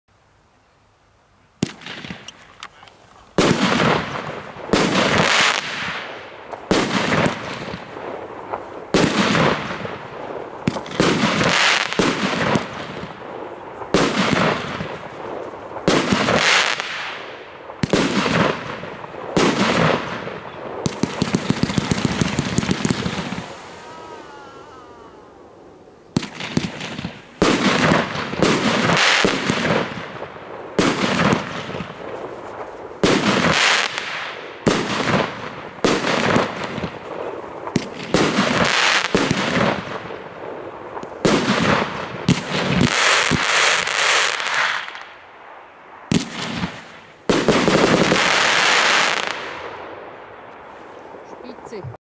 809-340 Symphonie of Colour
Gefächerte Kombinationsbatterie mit Buketts in Rot, Grün und Silber. Feuertöpfe in Gold und Silber mit blauen Sternen.